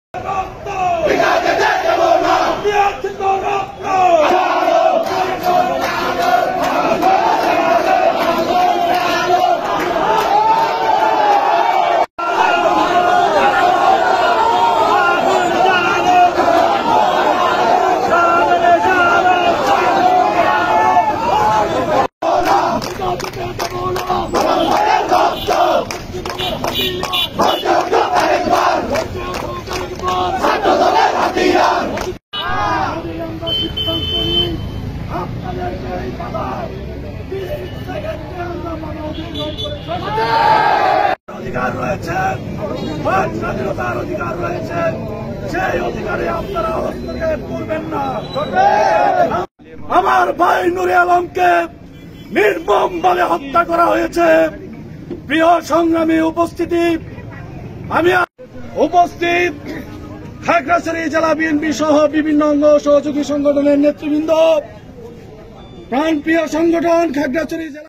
নুরে আলম এর মৃত্যুতে খাগড়াাছড়ি জেলা ছাত্রদলের তাৎক্ষণিক বিক্ষোভ মিছিল
৩ আগস্ট, ২০২২ বুধবার
পুলিশের গুলিতে মারাত্মক আহত হয়ে লাইফ সাপোর্টে শাহাদাত বরণ করা ভোলা জেলা ছাত্রদল সভাপতি নুরে আলম এর মৃত্যুতে খাগড়াাছড়ি জেলা ছাত্রদলের তাৎক্ষণিক বিক্ষোভ মিছিল ও প্রতিবাদ সমাবেশ।